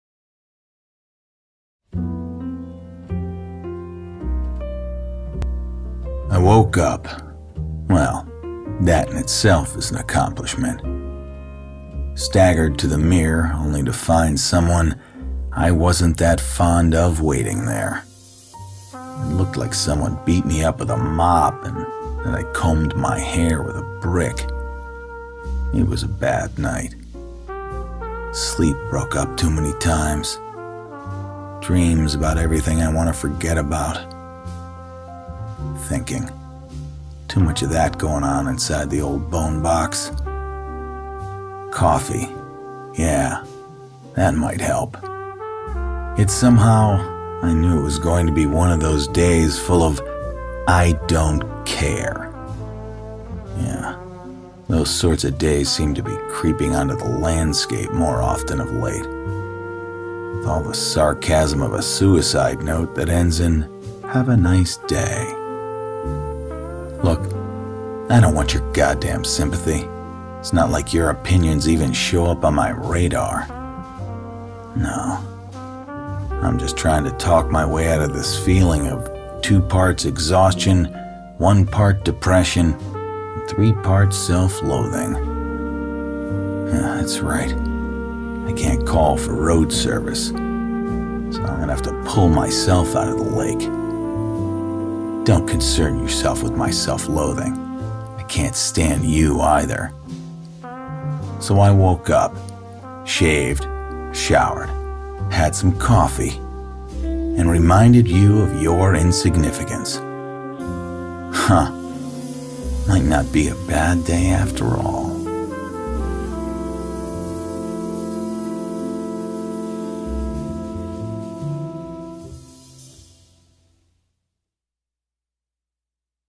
I WOKE UP
EYEZENMEDIA Presesnts a Roy Eisenstein Film

A Spoken Word Journey

Narrated by: David Cooley
Sound Design by: Tim Starnes
Written by: Roy Eisenstein